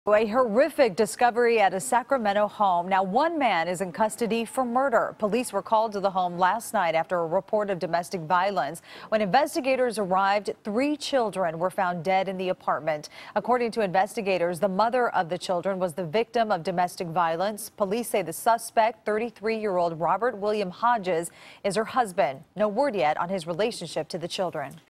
3 Kids Found Dead In West Sacramento Apartment, Man Arrested
Authorities responding to a domestic violence call in West Sacramento discovered three children who were dead Wednesday night. Police arrested a 33-year-old man who is also suspected of abusing their mother. (9/14/17)